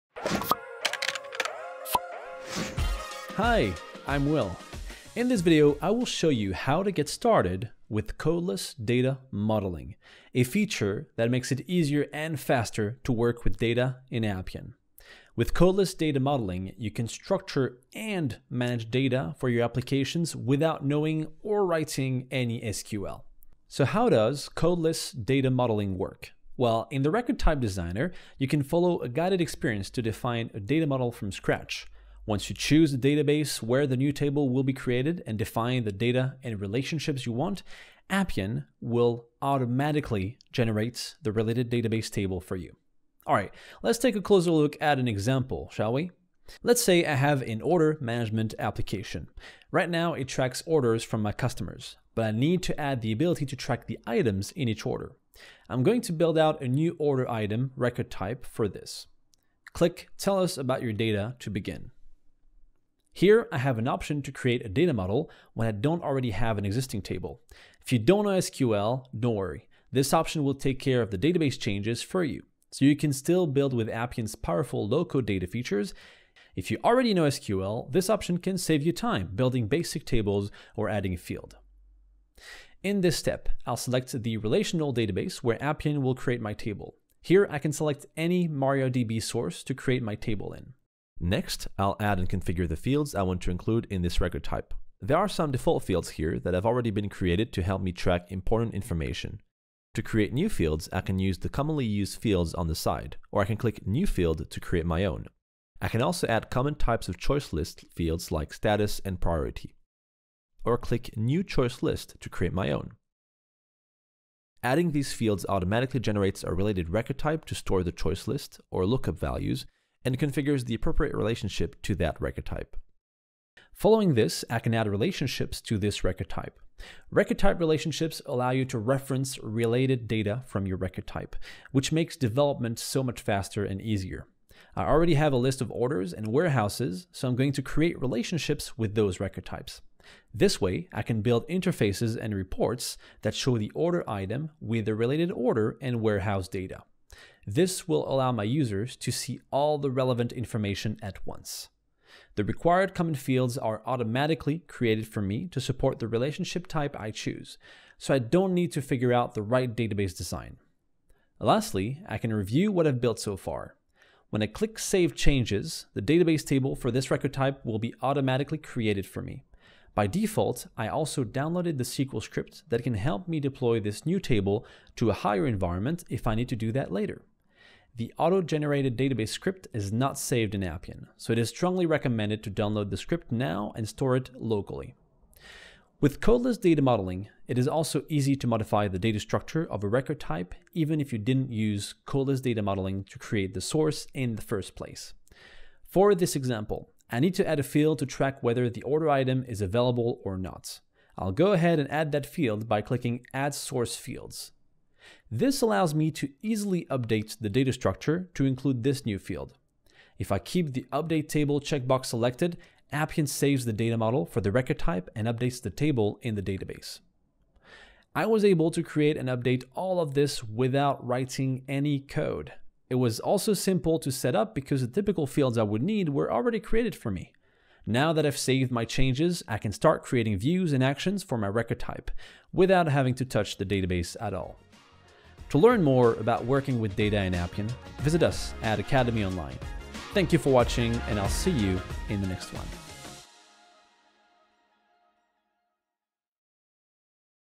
How to Structure and Manage Data Easily with Codeless Data Modeling
Learn how to create data models fast in Appian with codeless data modeling. This feature allows you to structure and manage data without knowing SQL.

Made on Appian 22.4